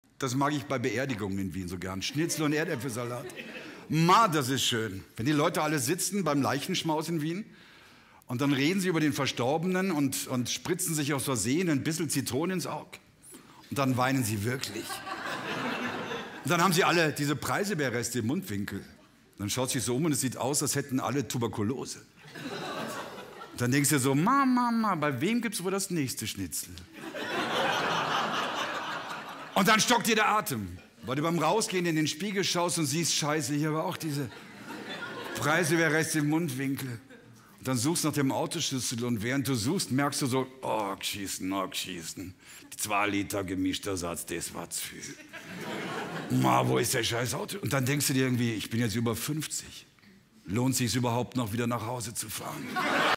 Beerdigungen in Wien | Dirk Stermann | BR Kabarett & Comedy
Dirk Stermann geht gerne auf Beerdigungen in Wien. Allein das Essen dort bereitet ihm große Freude. Doch die ganze Veranstaltung macht ihn auch nachdenklich, wie er im Vereinsheim Schwabing erzählt.